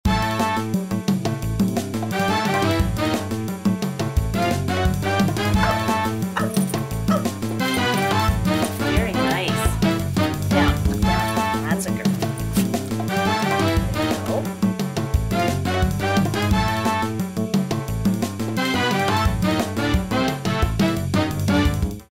Beautiful Intelligent Dog Sylvie Showing off her Talents for Talent Hounds Auditions.
What a friendly, beautiful, clever dog. Sylvie the Airedale came to the auditions for the Talent Hounds film and was such a star. Up and down, posing like a super model and having fun. So stylish, intelligent and talented. Talent Hounds film will air on The Pet Network. The documentary examines the evolving relationship between humans and dogs: from wolves to wonderkids.